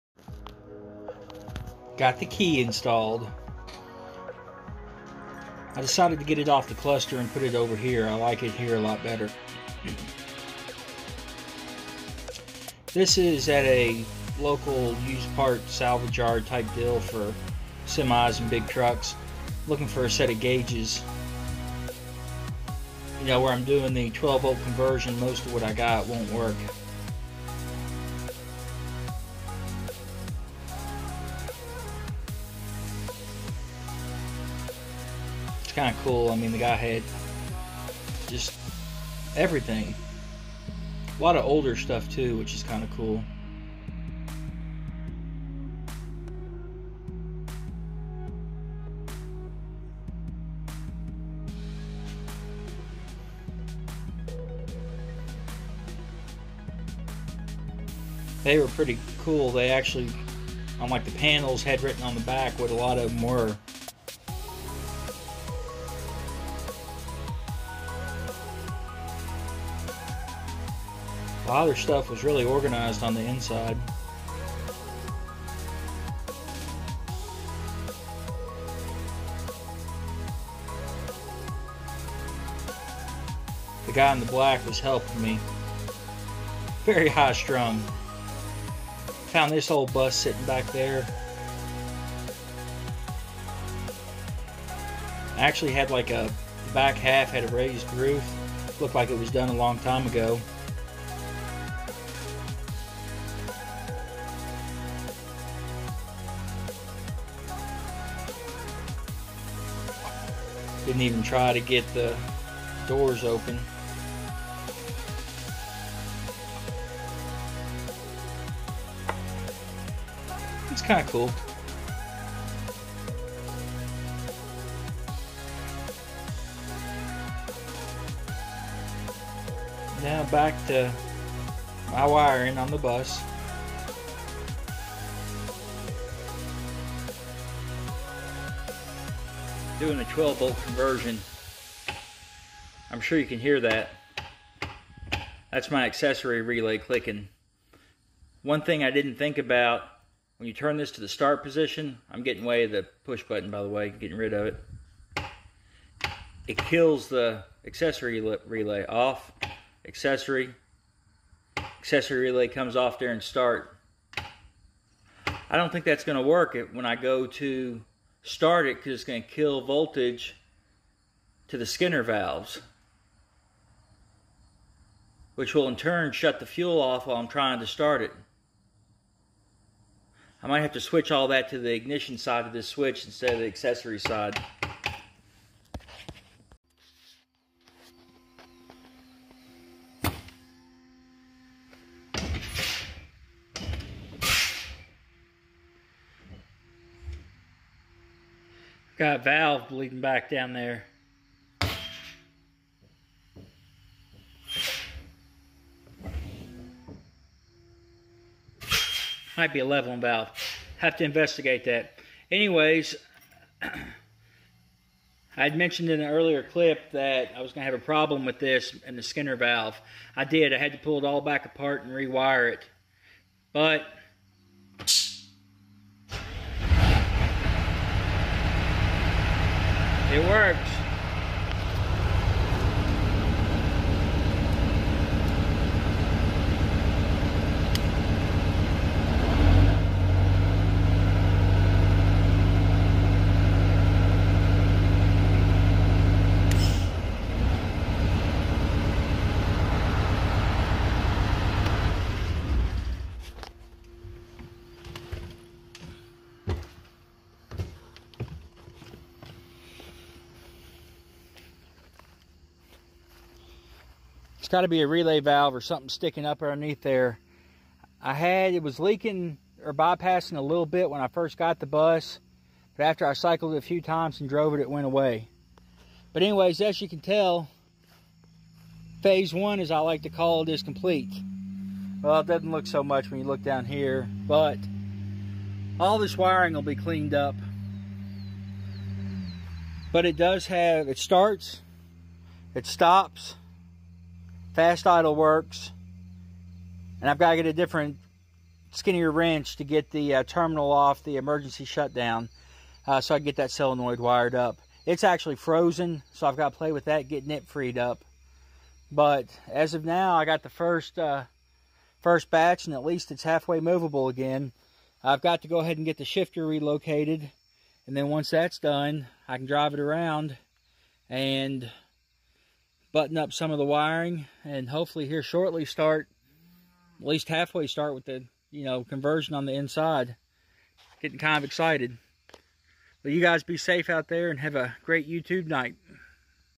MCI MC9 Bus Conversion - Stage 1 of the Rewire to 12v Complete
My 1980 MCI MC9 Ex Greyhound.  Soon to be an usable motor home I hope.